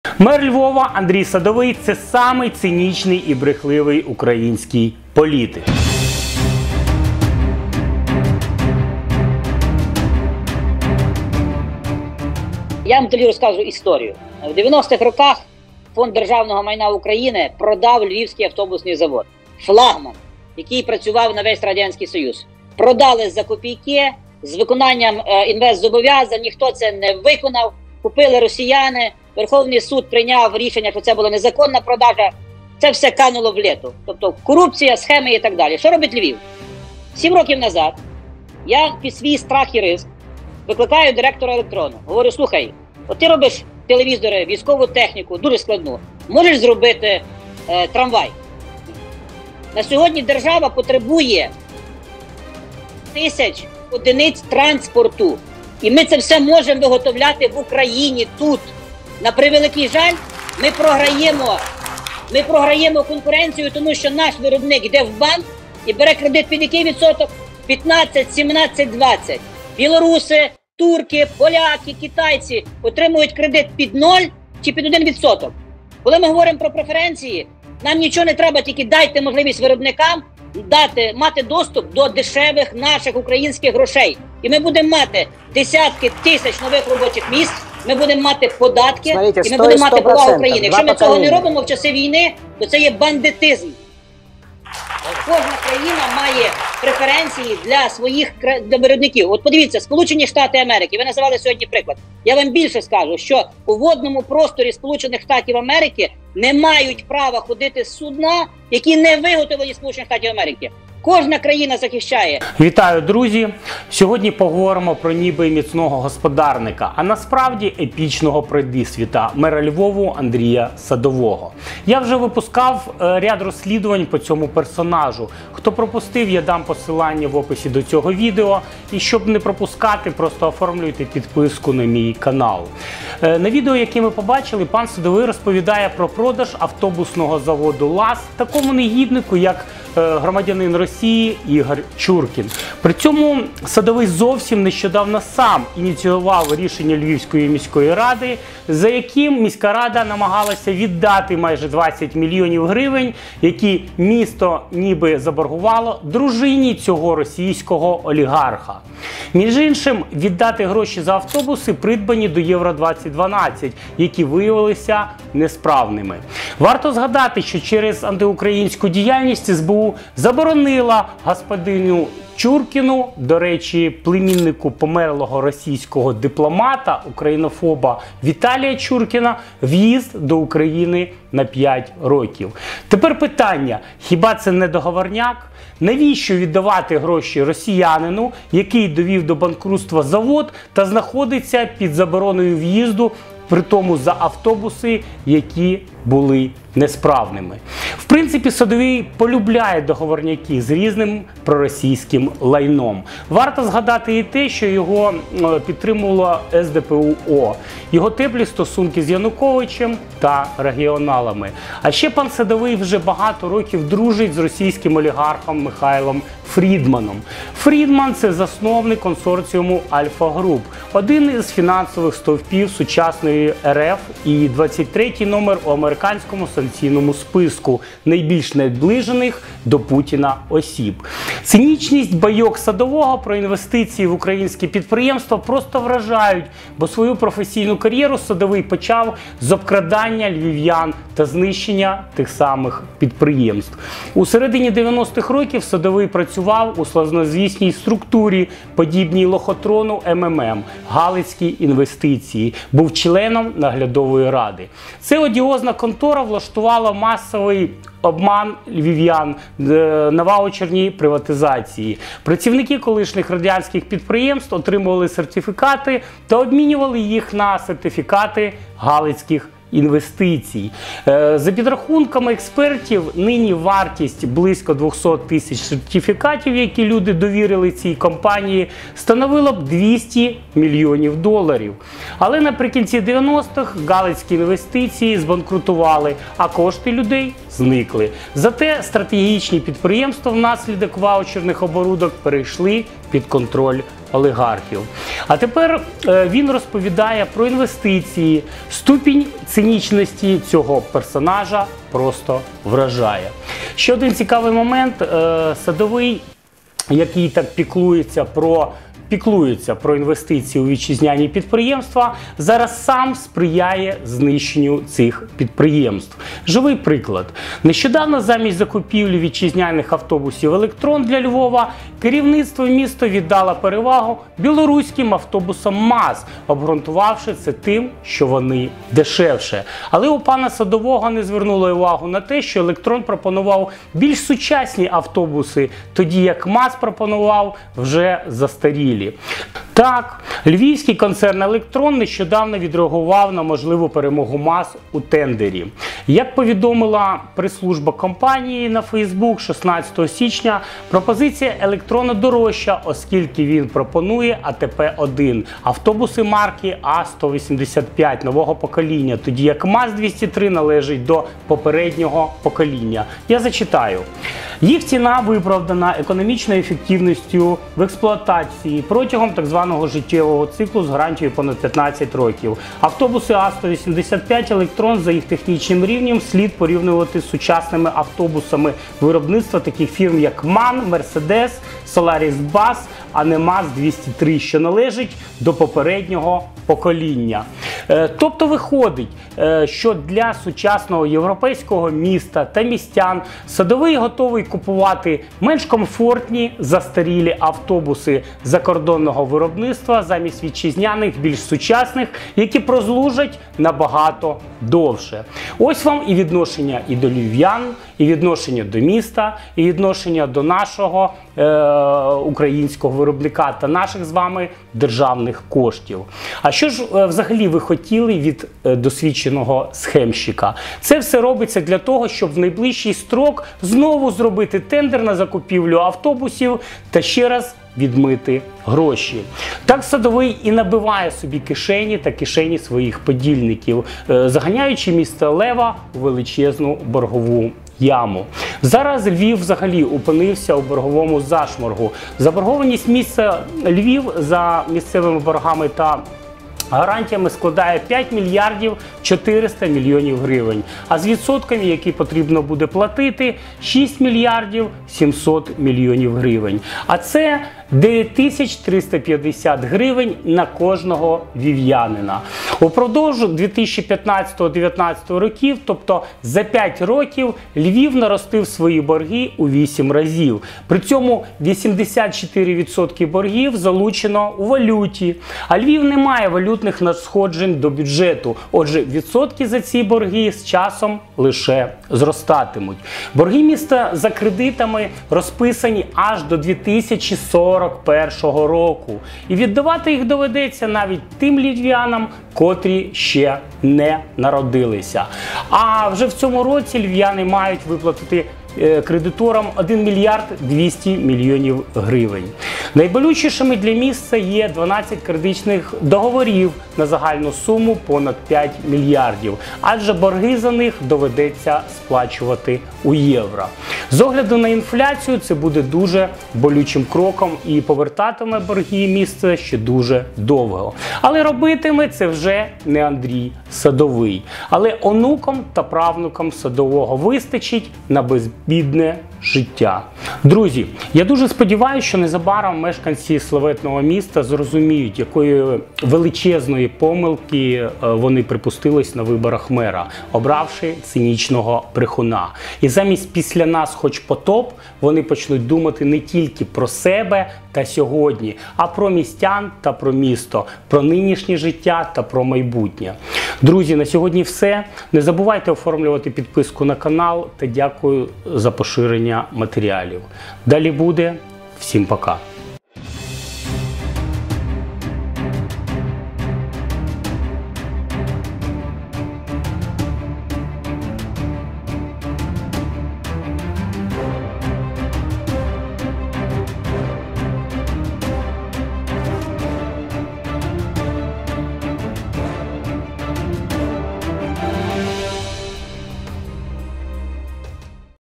Андрій Садовий - на словах за українського виробника на ділі за схеми та відкати
Мер Львова Андрій Садовий це самий цинічний і брехливий український політик.

У матеріалі:
- цинізм та брехня Садового у ефірі Шустера
- ініциатива Садового - віддати 20 мільйонів гривень дружині російського олігарха Ігоря Чуркіна
- Андрій Садовий та договорняки з російським "лайном"
- Андрій Садовий та російський олігарх Михайло Фрідман – засновник консорціуму «Альфа-груп», один із фінансових стовпів сучасної РФ і 23-й номер у американському санкційному списку найбільш наближених до Путіна осіб
- Андрій Садовий та лохотрон АО "МММ" для Львів'ян - «Галицькі інвестиції»
- Тендер: вітчизняні автобуси «Електрон» VS білоруські автобуси МАЗ - Садовий вбиває Українського виробника та фінансує білоруського диктатора за відкати
- борговий зашморг м. Львів - борги міста за кредитами розписані аж до 2041 року, упродовж 2015–2019 рр., тобто за 5 років Львів наростив свої борги у вісім разів
- цинічний брехун Андрій Садовий - величезна помилка на виборах мера